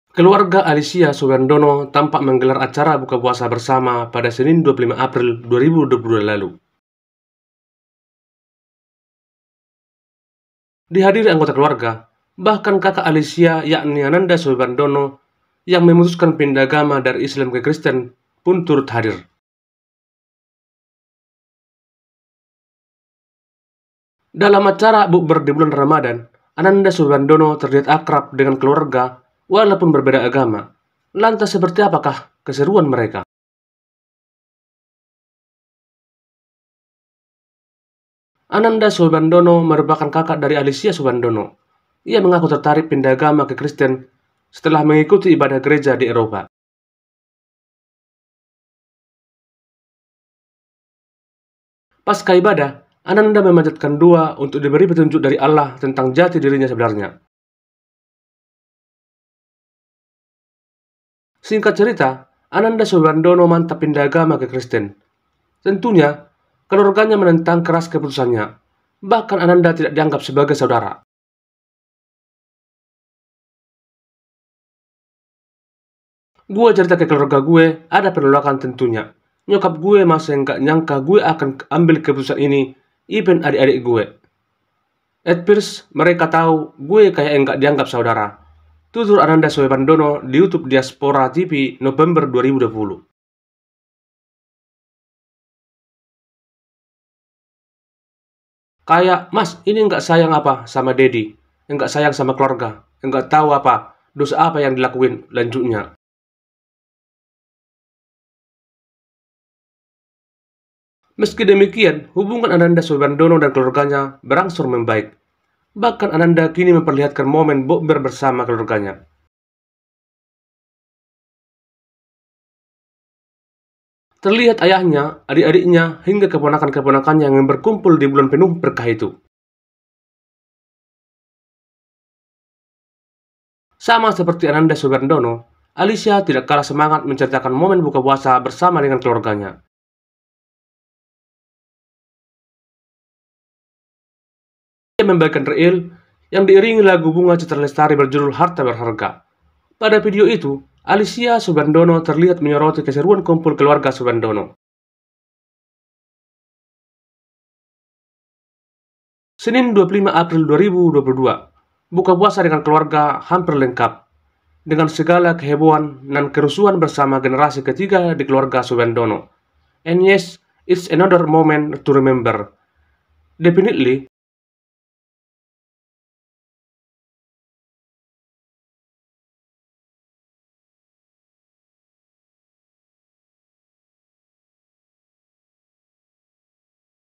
Pindah Agama Kristen, Momen Ananda Soebandono Bukber Buat Tersentuh
Keluarga Alyssa Soebandono tampak menggelar acara buka puasa bersama pada Senin, 25 April 2022 lalu.

Dihadiri anggota keluarga, bahkan kakak Alyssa yakni Ananda Soebandono yang putuskan pindah agama dari Islam ke Kristen turut hadir.

Dalam acara bukber di bulan Ramadhan, Ananda Soebandono terlihat akrab dengan keluarga walaupun berbeda agama. Lantas, seperti apa keseruan mereka?